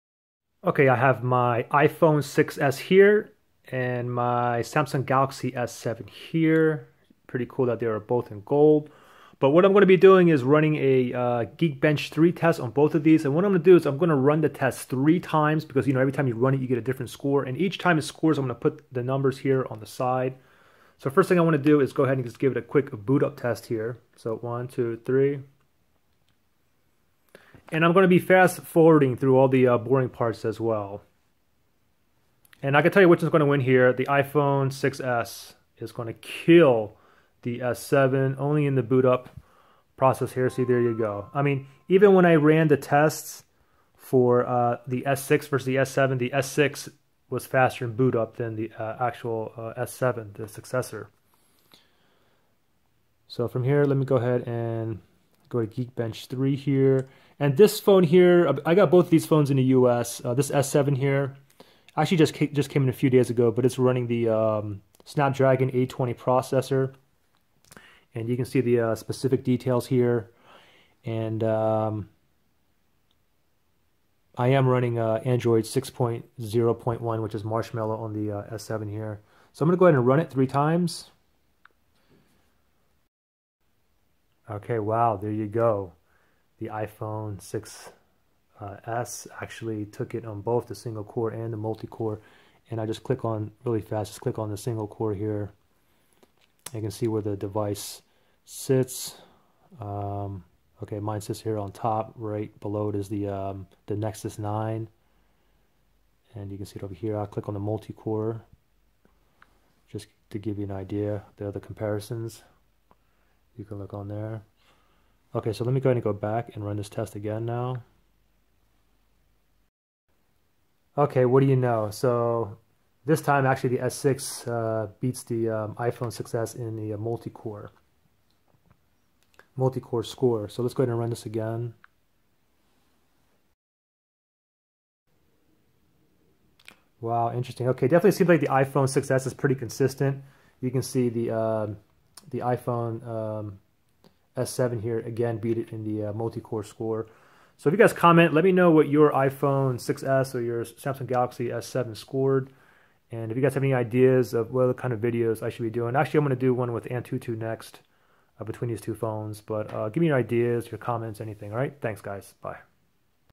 iPhone 6s vs Galaxy S7- GeekBench 3 Benchmark Test- Guess which phone is faster?
I run the Geekbench 2 benchmark test three times back to back to back as you know each time the score can be different. I run my Samsung Galaxy S7 against my iPhone 6s. I fast forwarded the boring parts so I don't waste your time.

Let me know your thoughts. What did your S7 or iPhone 6s score?
Let me know if you would like to see other testings or have suggestions to make these videos better. Thanks!